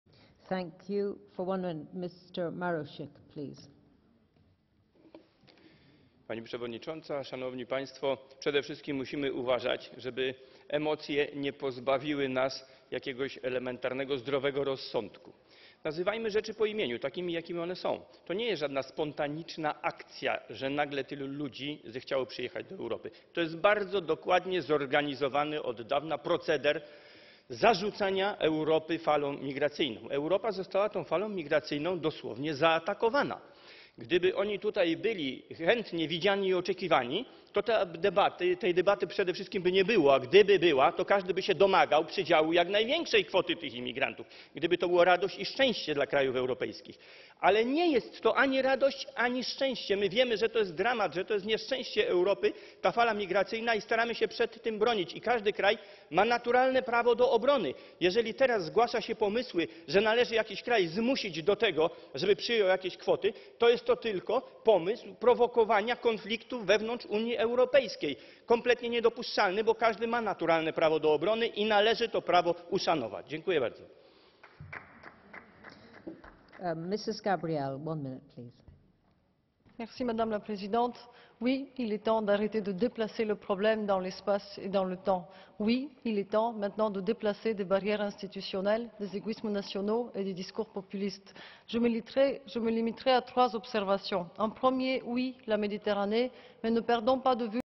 Wystąpienie Michała Marusika w Brukseli 16.09.2015r.
Na forum plenarnym Parlamentu Europejskiego odbyła się debata nt. imigrantów i uchodźców, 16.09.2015r.